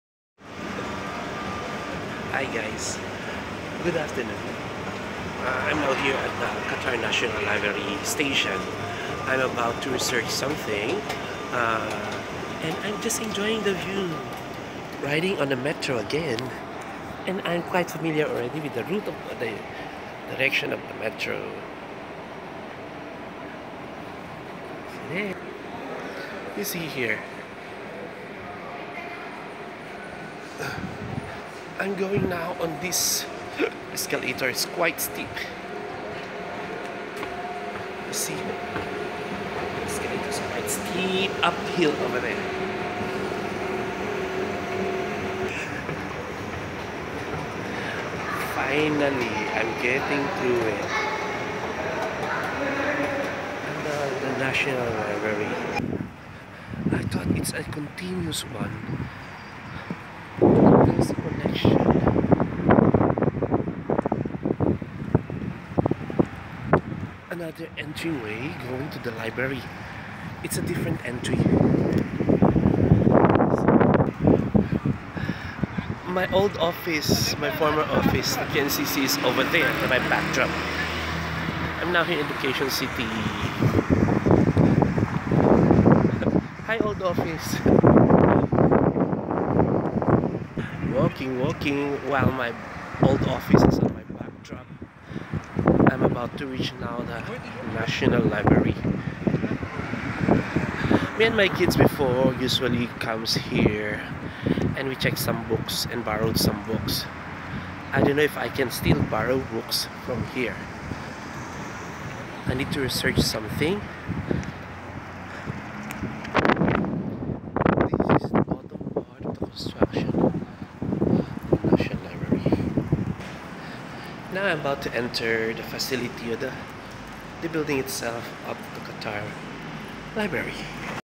Dad @ Metro Green to National Library
Thru Metro Green going to Qatar national Libraru in QF Education City.